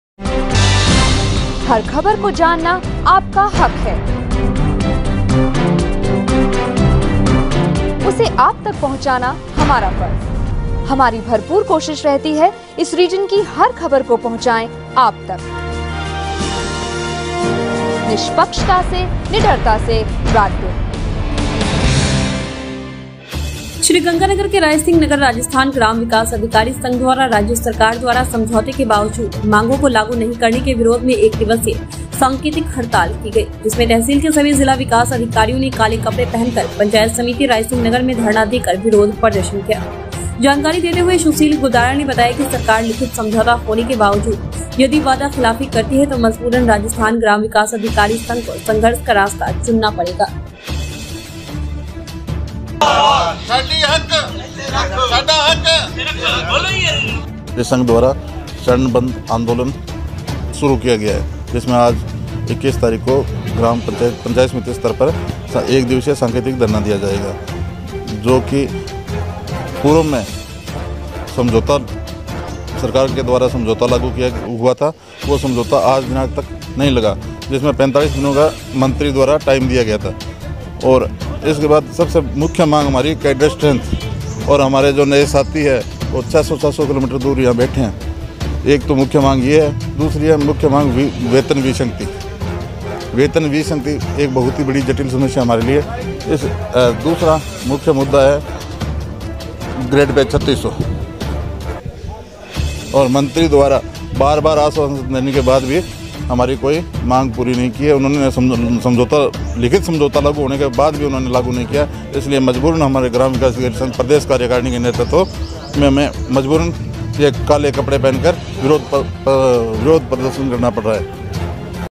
वादाखिलाफी को लेकर ग्राम विकास अधिकारी बैठे 1 दिन के सांकेतिक धरने पर
*रिपोर्टर आर एस कड़वा*

श्री गंगानगर के रायसिंहनगर राजस्थान ग्राम विकास अधिकारी संघ द्वारा राज्य सरकार द्वारा समझौते के बावजूद मांगों को लागू नहीं करने के विरोध में एक दिवसीय सांकेतिक हड़ताल की गई जिसमें तहसील के सभी जिला विकास अधिकारियों ने काले कपड़े पहन कर पंचायत समिति रायसिंहनगर में धरना देकर विरोध प्रदर्शन किया जानकारी देते हुए सुशील गोदारा ने बताया की सरकार लिखित समझौता होने के बावजूद यदि वादाखिलाफी करती है तो मजबूरन राजस्थान ग्राम विकास अधिकारी संघ को संघर्ष का रास्ता चुनना पड़ेगा

बाइट 1  *जिला मंत्री सुशील बिश्नोई*